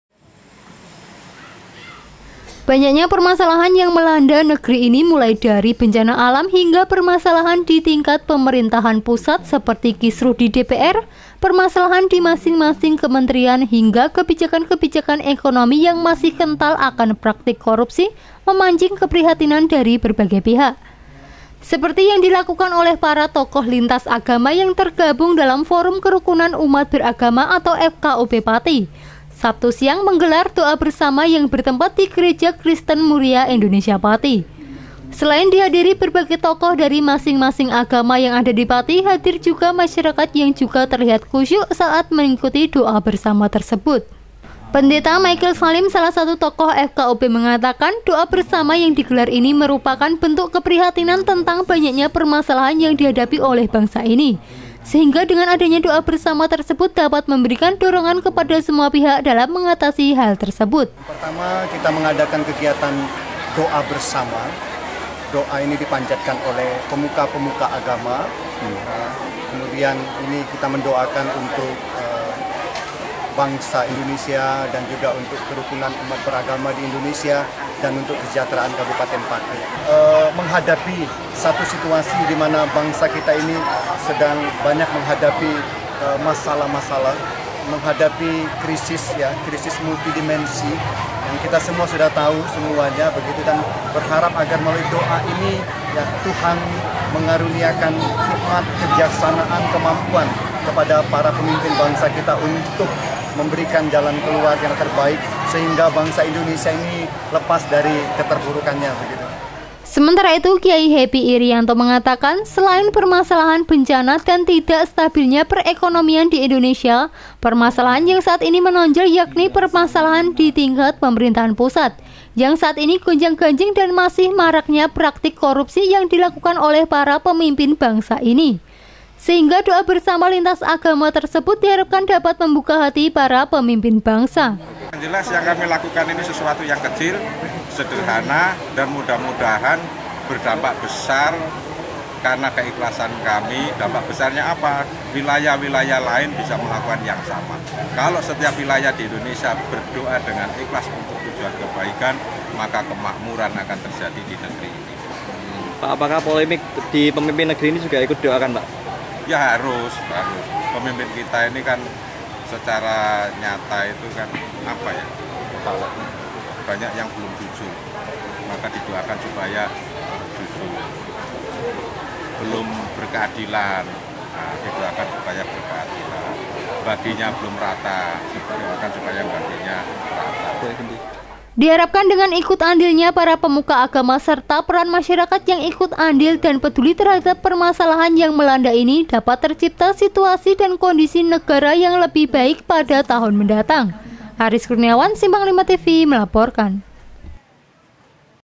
DO'A BERSAMA---BERITA 5 SIMPANG 5 TV---TGL 5
BANYAKNYA PERMASALAHAN YANG MELANDA NEGERI INI MULAI DARI BENCANA ALAM HINGGA PERMASALAHAN DI TINGKAT PEMERINTAHAN PUSAT SEPERTI KISRUH DI DPR/ PERMASALAHAN DI MASING-MASING KEMENTRIAN/ HINGGA KEBIJAKAN-KEBIJAKAN EKONOMI YANG MASIH KENTAL AKAN PRAKTIK KORUPSI/ MEMANCING KEPRIHATINAN DARI BERBAGAI PIHAK//

SEPERTI YANG DILAKUKAN OLEH PARA TOKOH LINTAS AGAMA YANG TERGABUNG DALAM FORUM KERUKUNAN UMAT BERAGAMA (FKUB) PATI/ SABTU SIANG MENGGELAR DOA BERSAMA YANG BERTEMPAT DI GEREJA KRISTEN MURIA INDONESIA PATI// SELAIN DIHADIRI BERBAGAI TOKOH DARI MASING-MASING AGAMA YANG ADA DI PATI/ HADIR JUGA MASYARAKAT YANG JUGA TERLIHAT KHUSYU/ DALAM MENGIKUTI DOA BERSAMA TERSEBUT//

PENDETA MAYKEL SALIM SALAH SATU TOKOH FKUB MENGATAKAN/ DOA BERSAMA YANG DIGELAR MERUPAKAN BENTUK KEPRIHATINAN TENTANG BANYAKNYA PERMASALAHAN YANG DIHADAPI OLEH BANGSA INI// SEHINGGA DENGAN ADANYA DOA BERSAMA TERSEBUT DAPAT MEMBERIKAN DORONGAN KEPADA SEMUA PIHAK DALAM MENGATASI HAL TERSEBUT//

WAWANCARA   : MICHAEL SALIM  (TOKOH FKUB)

SEMENTARA ITU/ KYAI HEPPY IRIYANTO MENGATAKAN/ SELAIN PERMASALAHAN BENCANA DAN TIDAK STABILNYA PEREKONOMIAN DI INDONESIA/ PERMASALAHAN YANG SAAT INI MENONJOL YAKNI PERMASALAHAN DITINGKAT PEMERINTAHAN PUSAT/ YANG SAAT INI GONJANG-GANJING/ DAN MASIH MARAKNYA PRAKTIK KORUPSI YANG DILAKUKAN OLEH PARA PEMIMPIN BANGSA INI// SEHINGGA DOA BERSAMA LINTAS AGAMA TERSEBUT DIHARAPKAN DAPAT MEMBUKA HATI PARA PEMIMPIN BANGSA//

WAWANCARA   : KYAI HEPPY IRIANTO  (TOKOH FKUB)

DIHARAPKAN/ DENGAN IKUT ANDILNYA PARA PEMUKA AGAMA SERTA PERAN MASYARAKAT YANG IKUT ANDIL DAN PEDULI TERHADAP PERMASALAHAN YANG MELANDA INI/ DAPAT TERCIPTA SITUASI DAN KONDISI NEGARA YANG LEBIH BAIK PADA TAHUN MENDATANG//

ARIS KURNIAWAN/ SIMPANG5 TV MELAPORKAN//